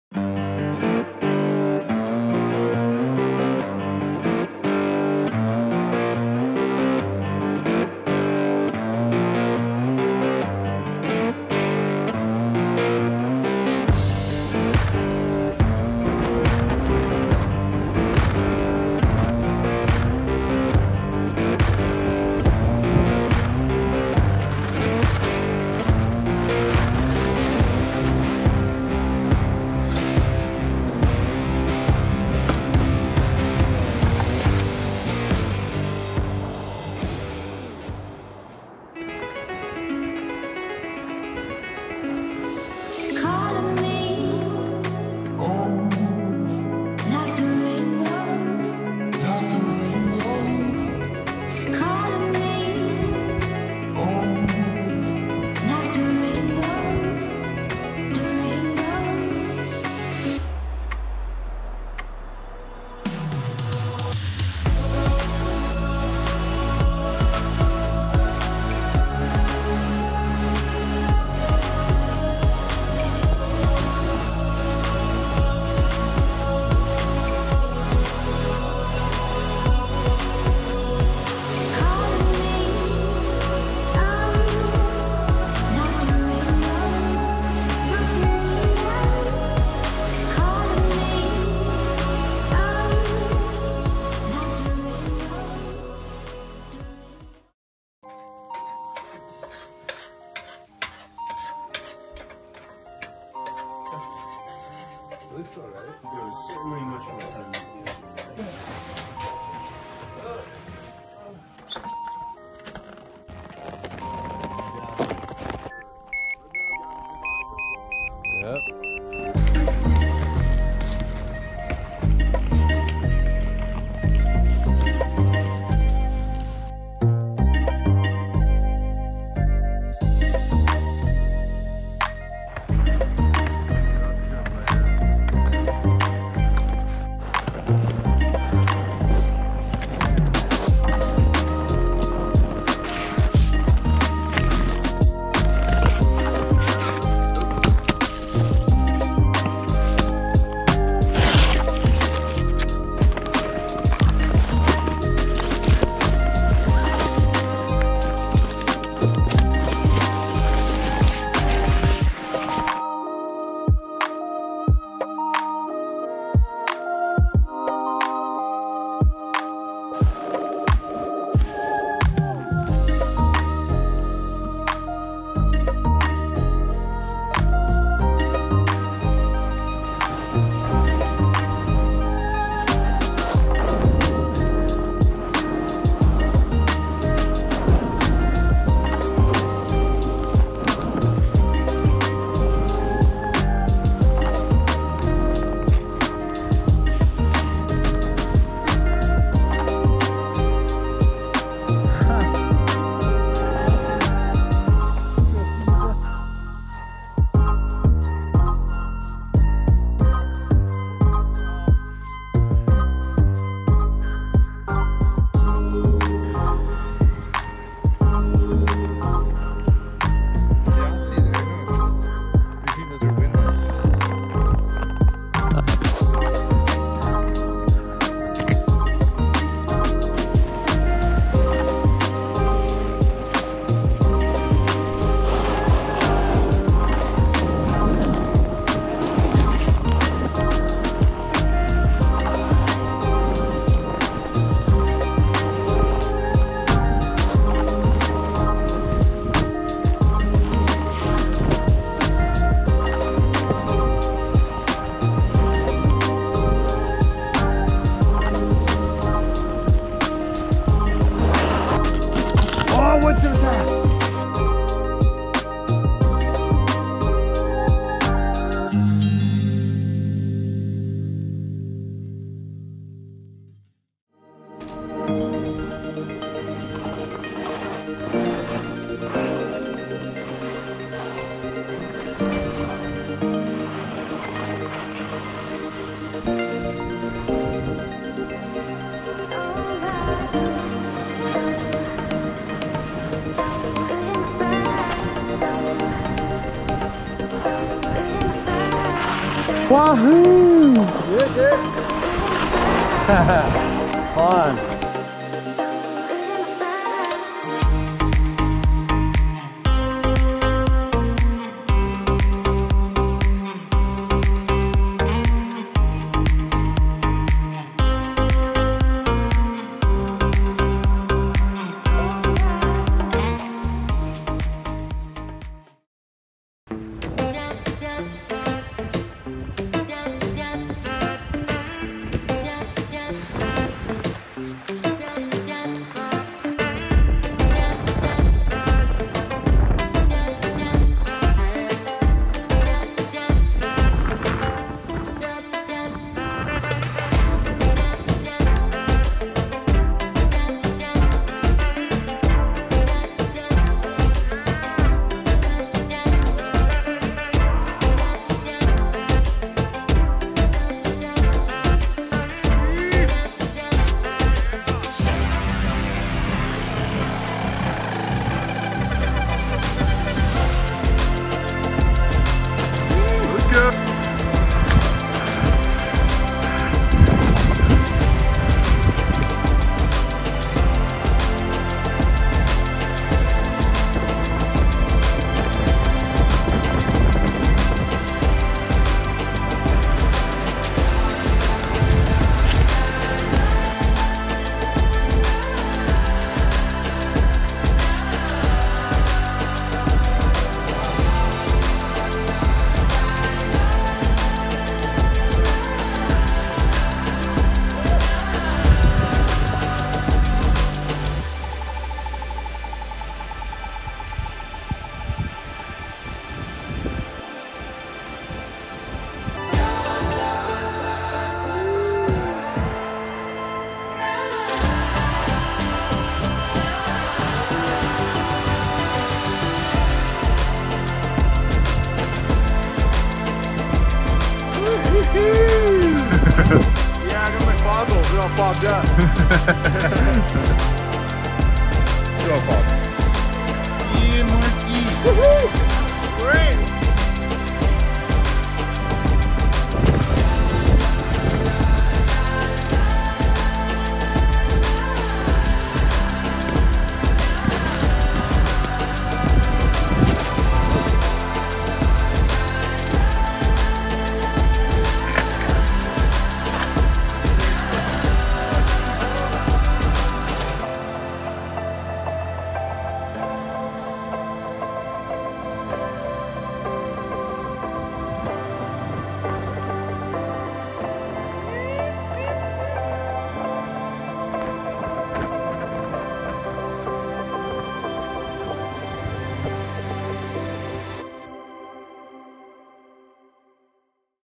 Three Sisters Backcountry Skiing
This is a short video of a three-day backcountry ski trip I did with some friends in the Three Sisters area of the Oregon Cascade Range. The dates of the trip were February 5-7, 2023. Gavin and Terrance have been doing this trip to the Tam Rim Alpine Yurts with Three Sisters Backcountry for years, and this is the second year I have joined them. On the east side of the Three Sisters Wilderness, Tam McArthur Rim rises 1,500 feet above the yurts and stretches for two miles, providing an incredible variety of terrain including open bowls, treed glades, couloirs, gentle meadows, and steep chutes. Beyond Tam McArthur Rim, the wilderness stretches over spectacular ridges, glades, lakes, and buttes leading up to Broken Top Mountain.